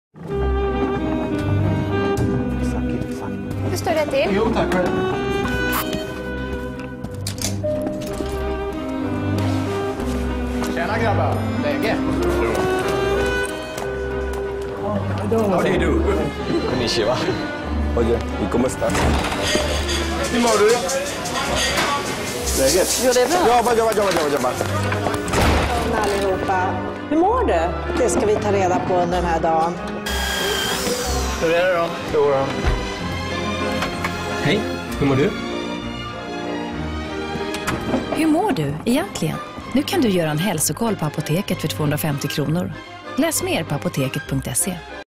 Reklam - Apotekets Hälsokoll
Here is my lastest film project. This is an ad for the national pharmacy system, Apoteket, here adversiting a health check up. I play the hotel manager on the cellphone. Recorded 2010.